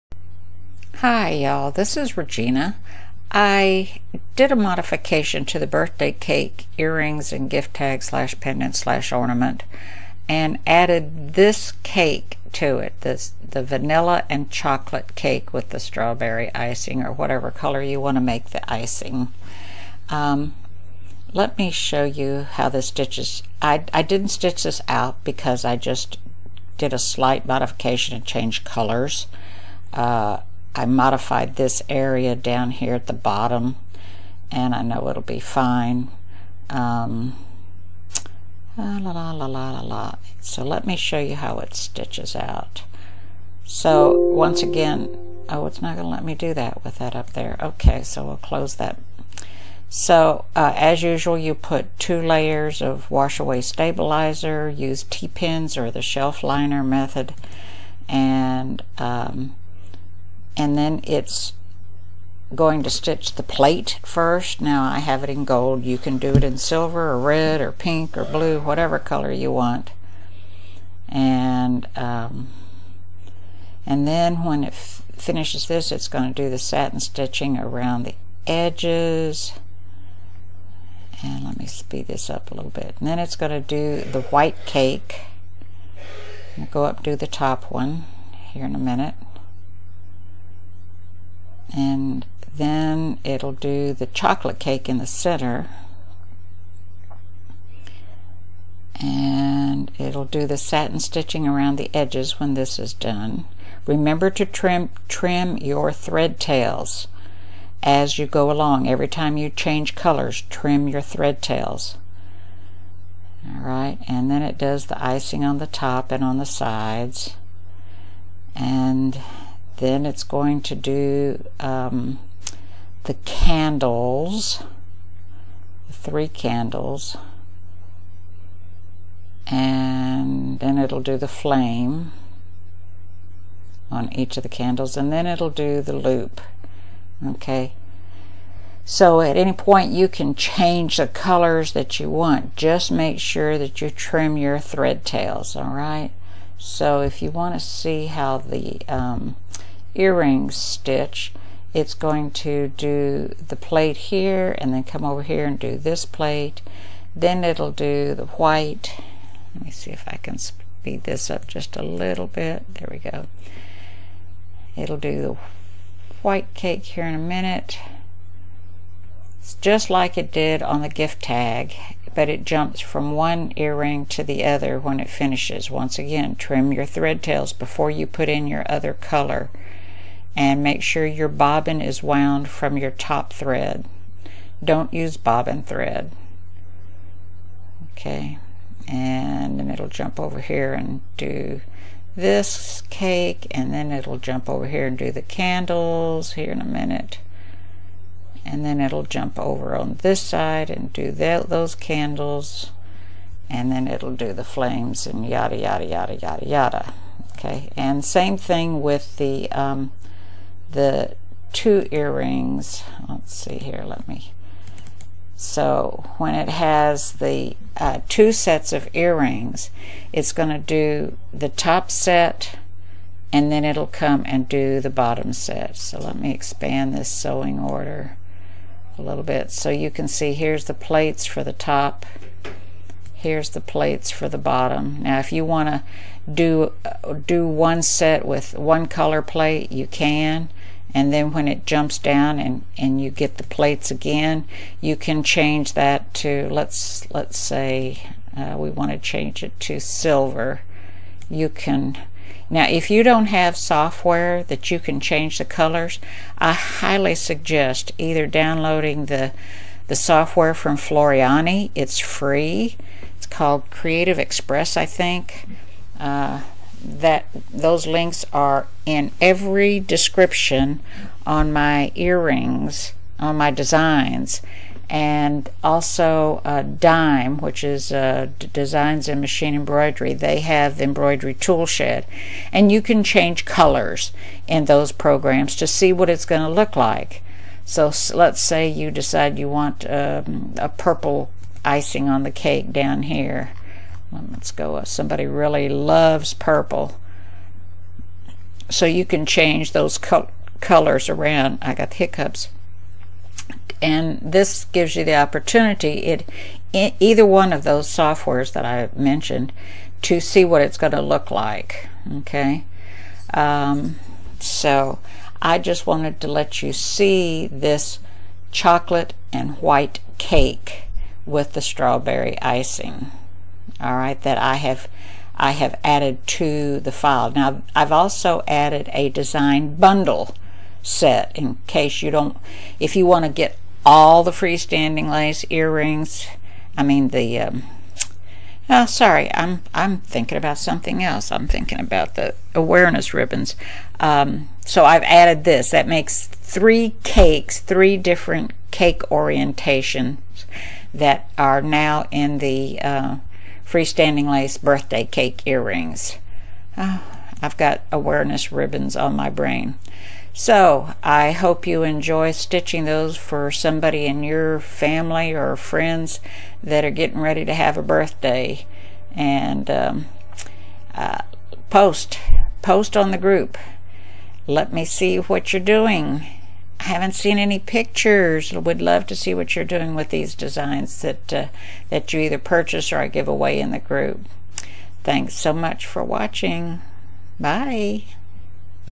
FSL Birthday Cake Chocolate and White Cake Earrings and Gift Tag/Pendant/Ornament
This is a stitch-out demo for the White and Chocolate Cake Earrings and Gift tag that I've added to the zip file in my ETSY SHop -  Now there are 3 different configurations of the  birthday cake sets in the zip file